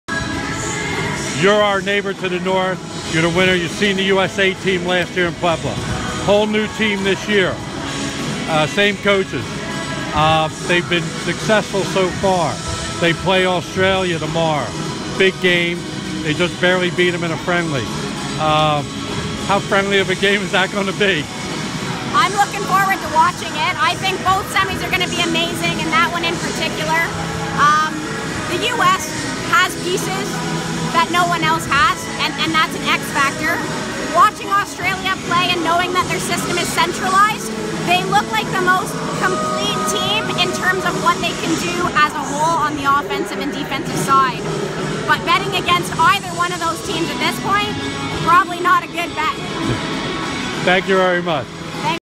Canada FIBA u17 Coach Jodi Gram / France 4
Canada FIBA u17 Coach Jodi Gram speaks about USA vs Australia FIBA u17s semifinal game.